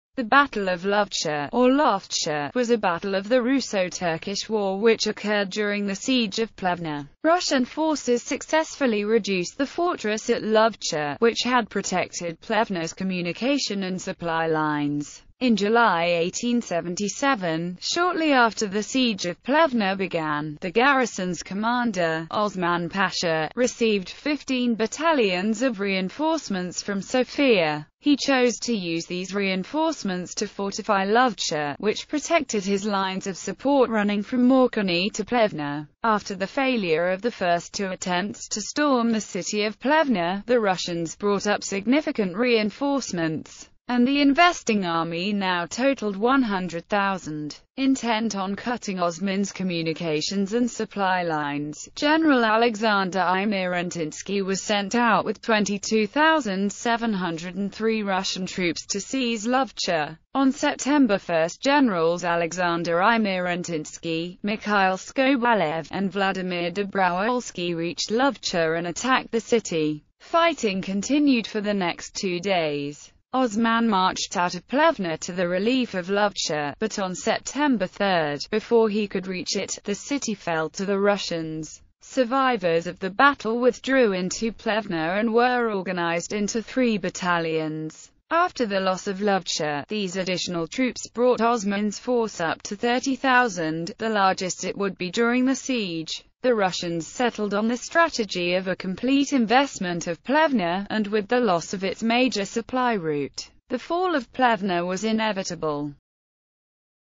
Battle of Lovcha
The Battle of Lovcha, or Loftcha (today Lovech), was a battle of the Russo-Turkish War (1877–1878) which occurred during the siege of Plevna.

Image-Copyright-Info
Image is in public domain
Author-Info:   Unknown
Image-Copyright-Info